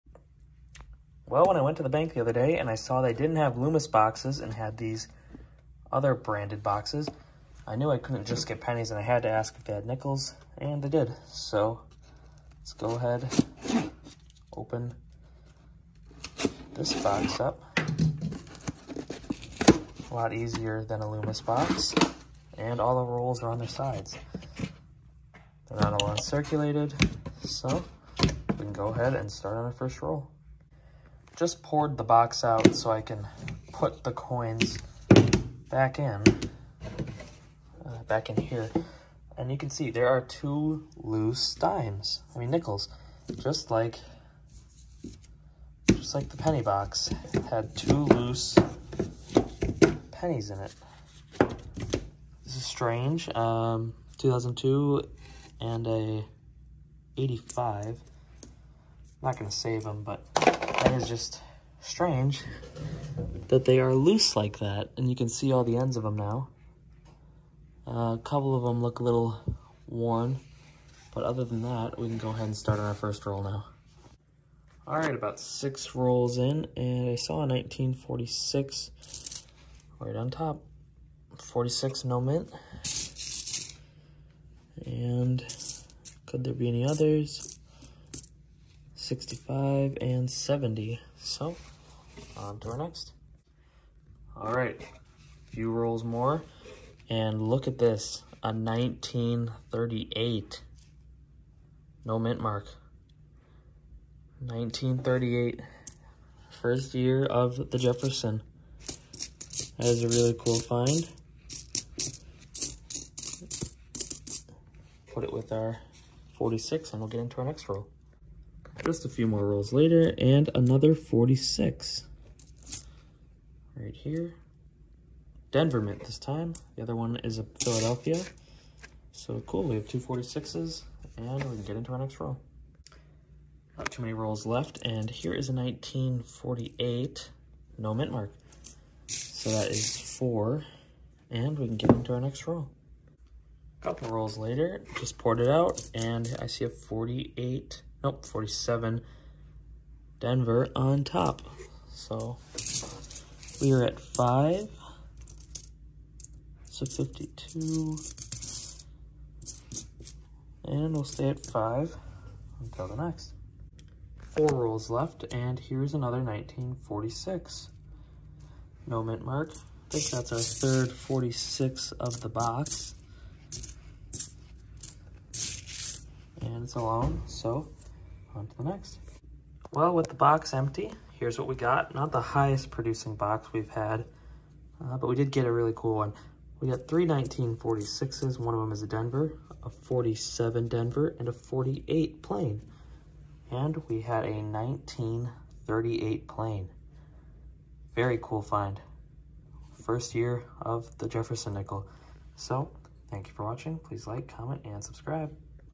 1938 JEFFERSON NICKEL! -Coin Roll Hunting-
In this video, I will be searching through a $100 box of nickels looking for V nickels, buffaloes, old Jefferson’s, silver, foreigns, and other cool coins!